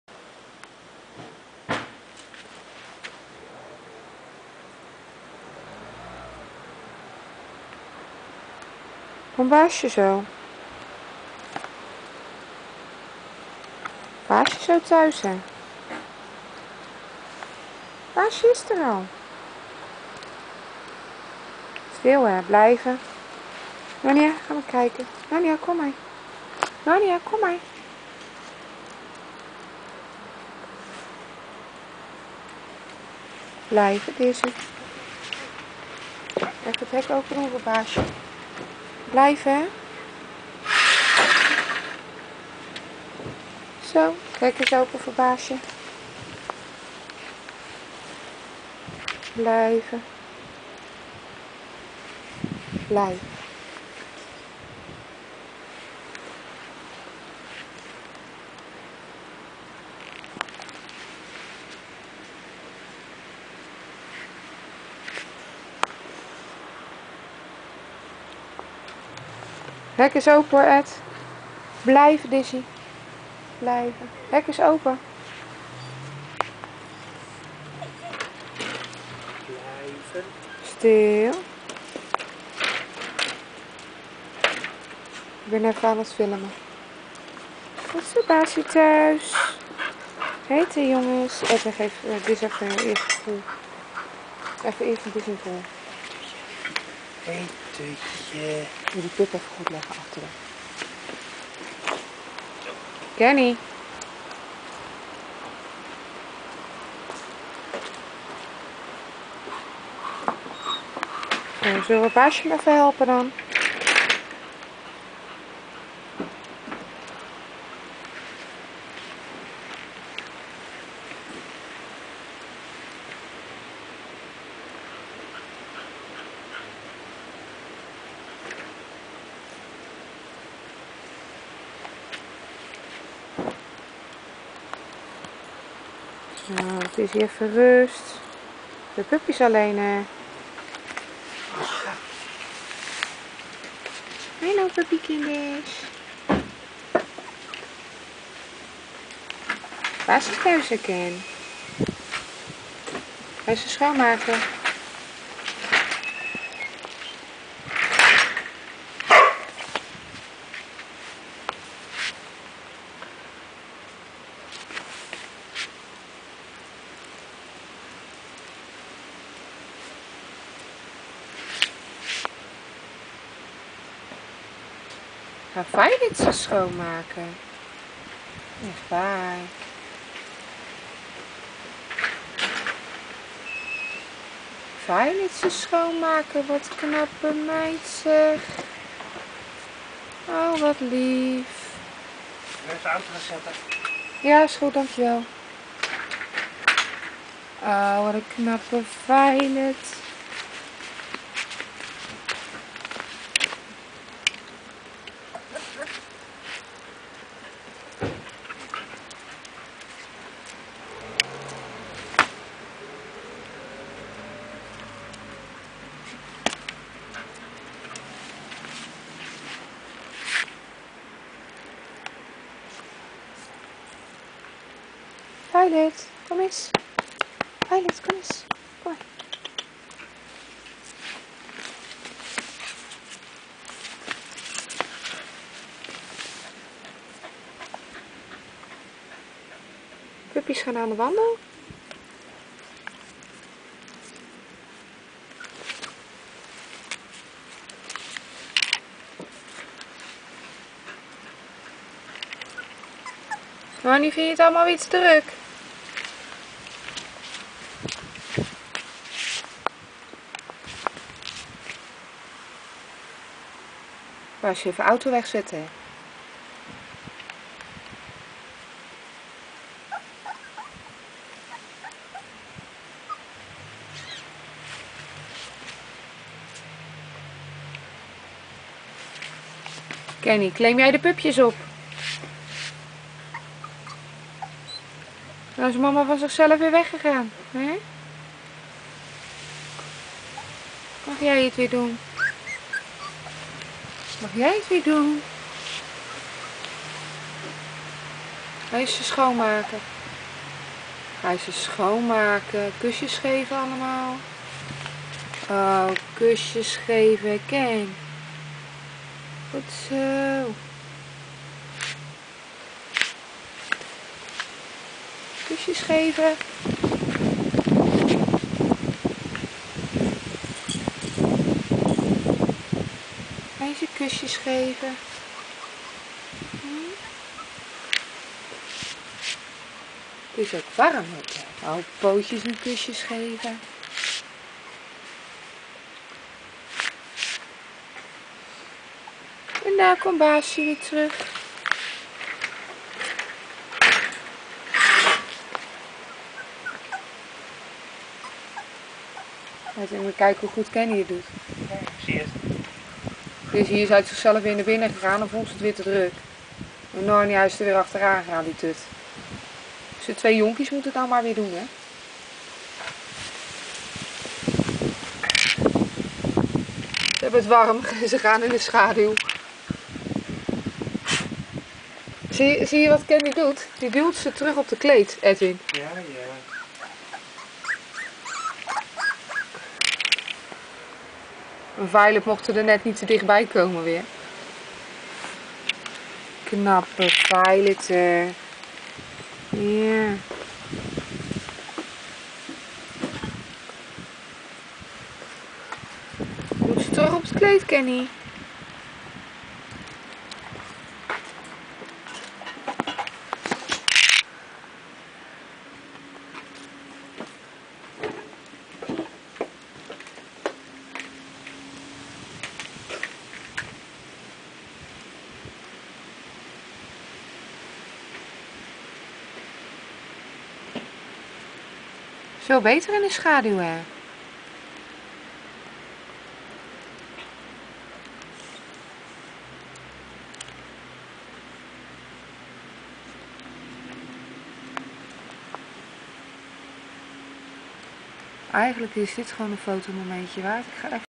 Puppy tweeling zalig buiten met BFJ famillie 12 Augustus
Mooi weer niet te heet en droog, puppy tweeling zalig naar buiten in de voortuin op bijna 3 weken oud, pups hebben altijd vanaf het begin een "veiligheid have baby kleed", waar ze altijd terug kunnen wat bekent is.